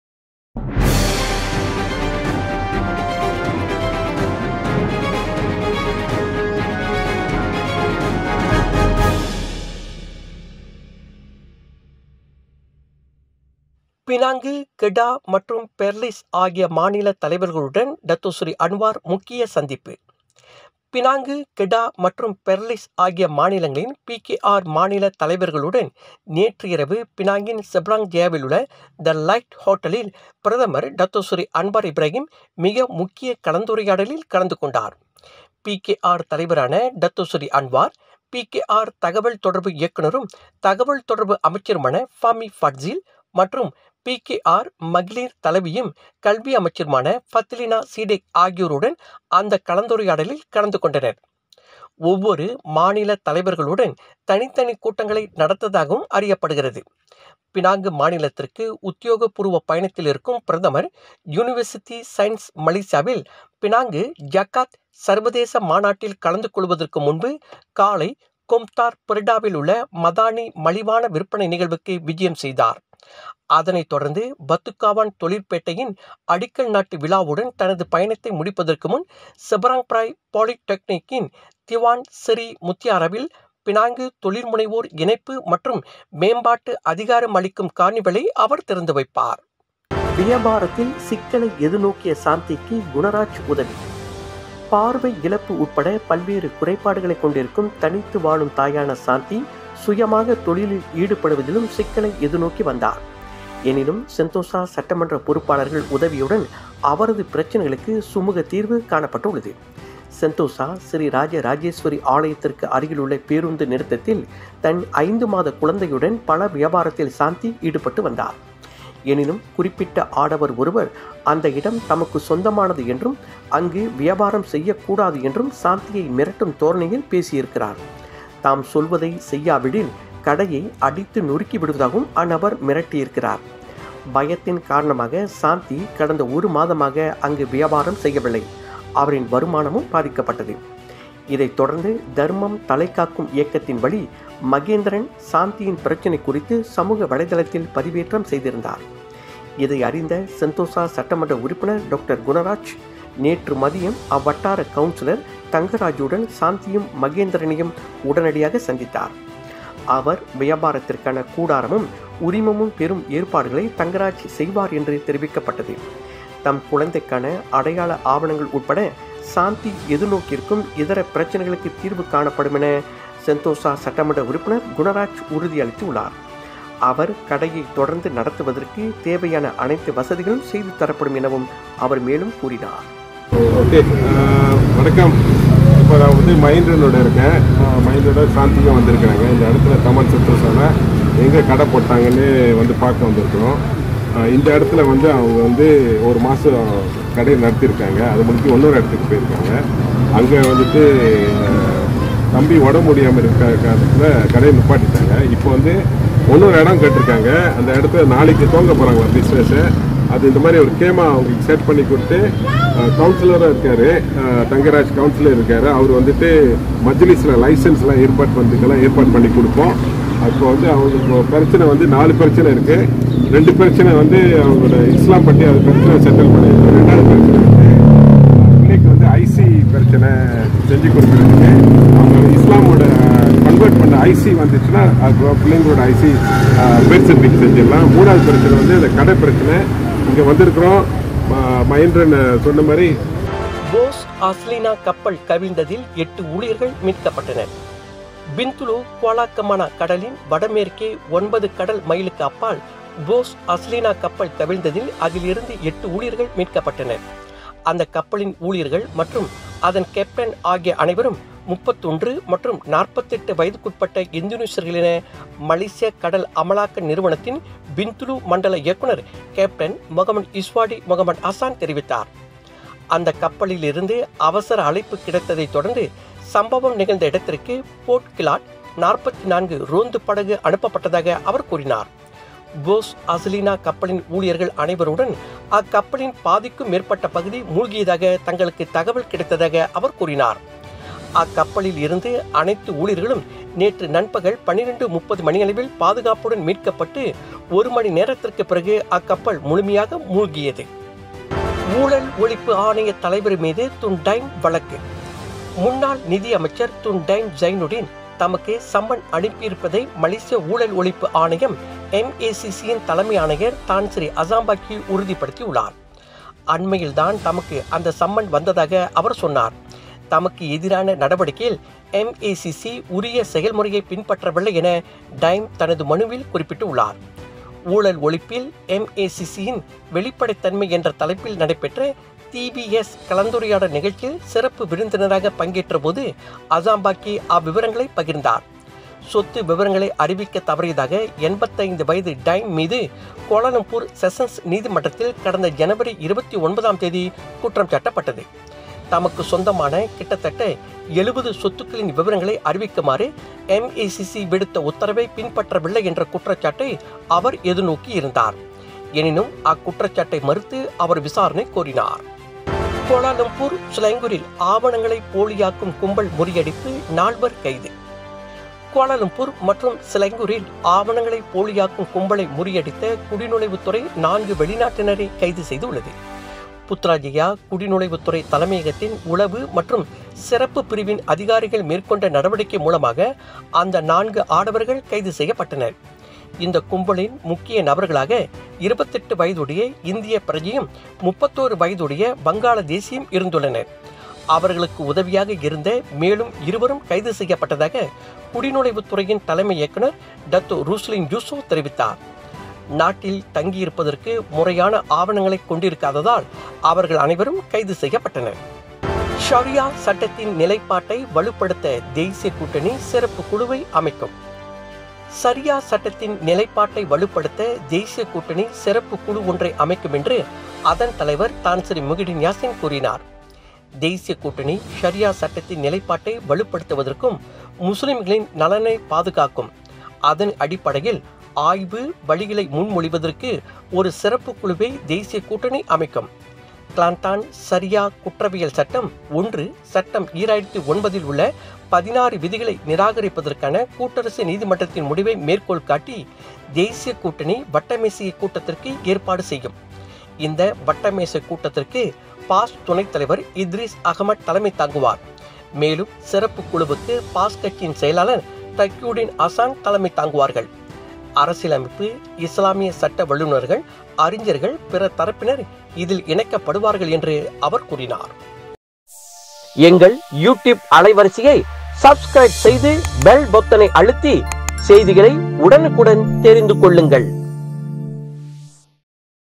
🔴 WATCH NOW | AST News | மூடிய அறையில் நீண்ட நேர கலந்துரையாடல்! அன்வாரின் அடுத்தகட்ட நடவடிக்கை!
✪ வியாபாரத்தில் சிக்கலை எதிர்நோக்கிய சாந்திக்கு மாண்புமிகு டாக்டர் குணராஜ் உதவி!

✪  ஊழல் ஒழிப்பு ஆணையத் தலைவர் மீது துன் டாய்ம் வழக்கு

✪ கோலாலம்பூர், சிலாங்கூரில் ஆவணங்களை போலியாக்கும் கும்பல் முறியடிப்பு; நால்வர் கைது